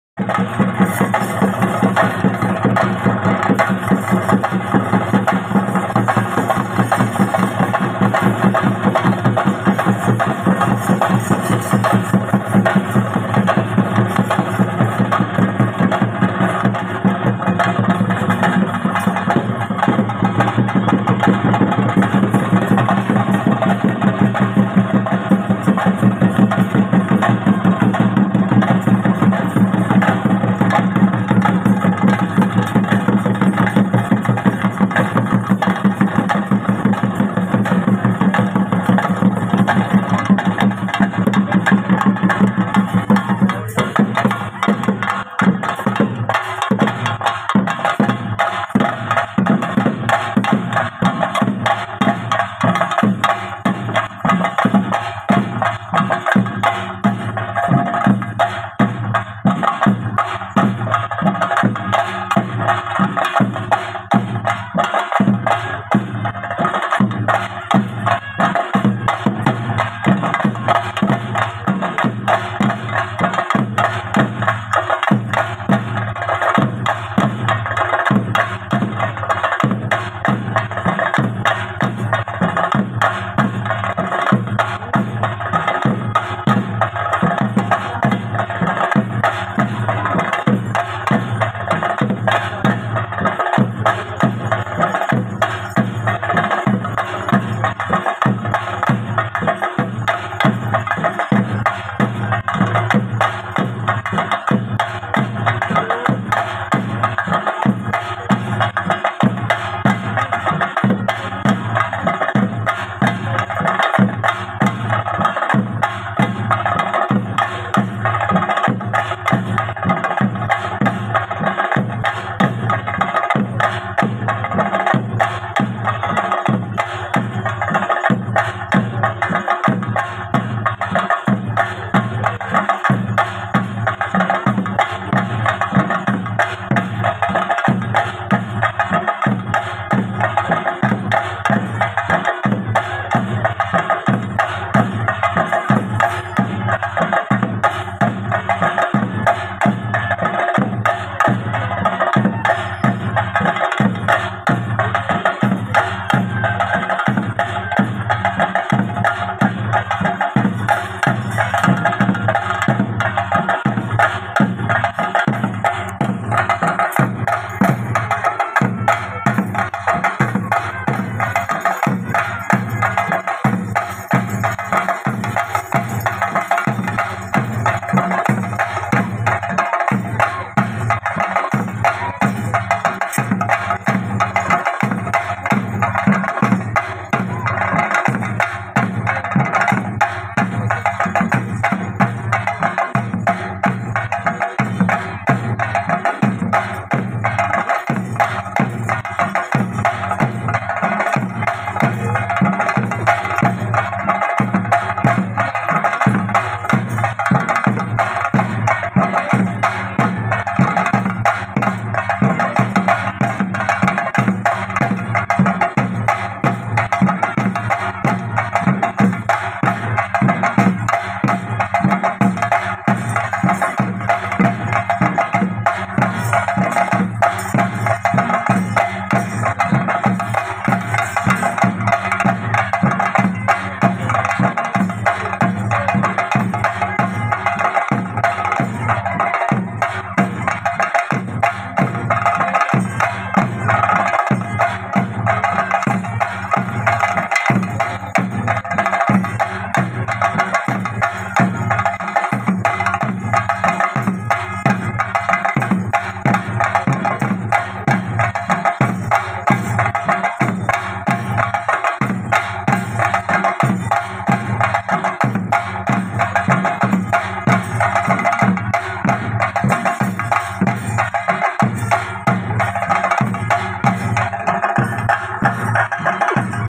ഭക്തരുടെ കണ്ണും മനസ്സും നിറയ്ക്കുന്ന വീരന്‍...!!!!കതിവന്നൂര്‍ വീരന്‍...!!!! 2024
തിലാന്നൂര്‍ ശ്രീ മന്ദപ്പന്‍ ക്ഷേത്രം
കോലധാരി : വിനു പെരുവണ്ണാന്‍ കണ്ടോന്താര്‍